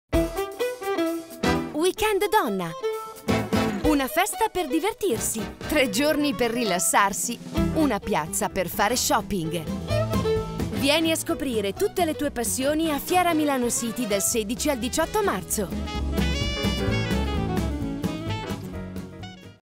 Weekend Donna 2012 -Fieramilanocity dal 16 al 18 Marzo
Lei tv sarà Media partner della fiera Weekend Donna a Fieramilanocity dal 16 al 18 Marzo, unica fiera totalmente dedicata alle passioni delle donne.
Dove ci sono le donne e le loro passioni Lei c'è.
Presente per la seconda volta all'evento, il nostro canale avrà uno stand dedicato dove faremo i casting per la nuova edizione di Giardinieri in affitto.
Per l'occasione il nostro stand sarà trasformato in un'oasi verde dove rilassarsi e cogliere l'occasione per diventare protagonista del programma!
Ti aspettiamo!

Giardinieri in affitto
dal 17 Aprile alle 21.00 su Lei canale 127 Sky.